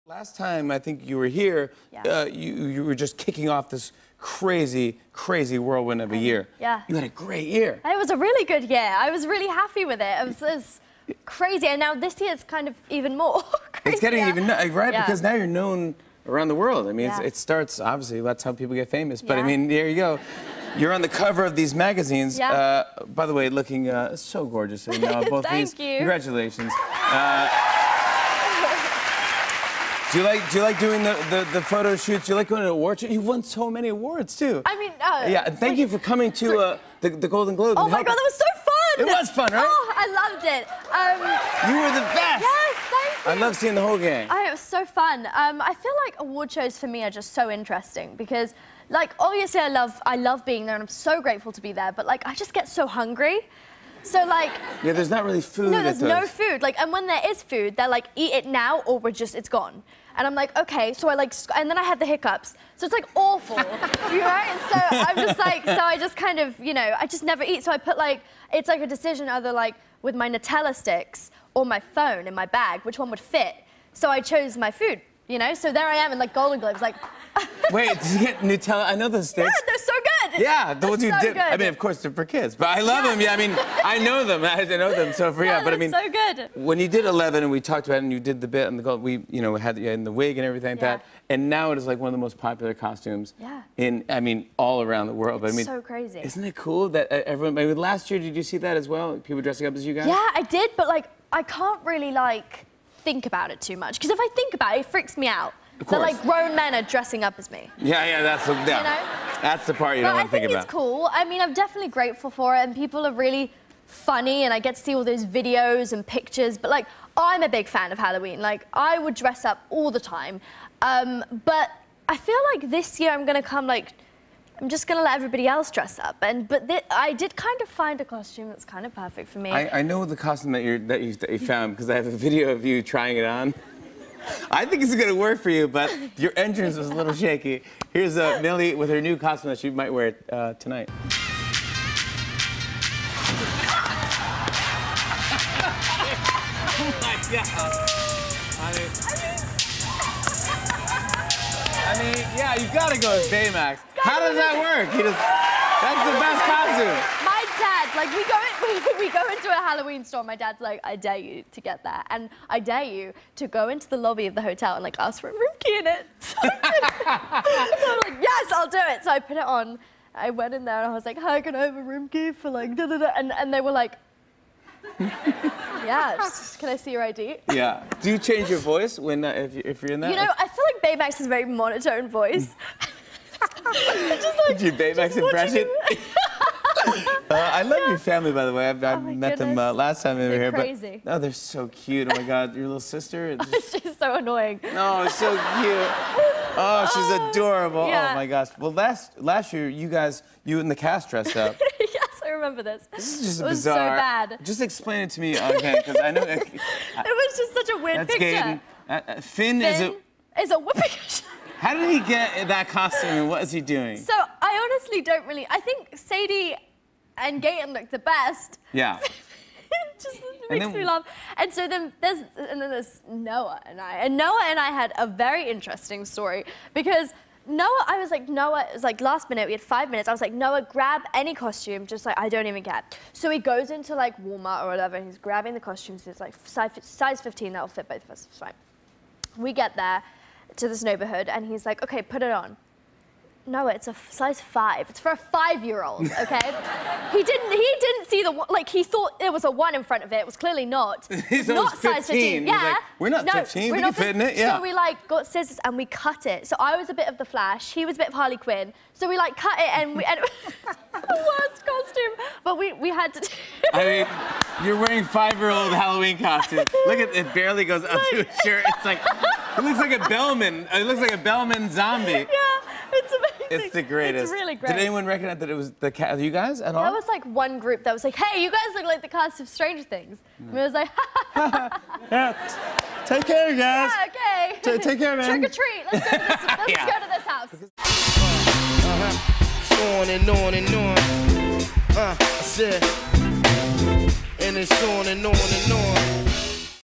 Millie Bobby Brown Is Freaked Out by Grown Men Dressing Up as Eleven
Millie Bobby Brown talks to Jimmy about her love for Halloween and explains how the stars of Stranger Things ended up trick-or-treating in wonky-looking super hero costumes.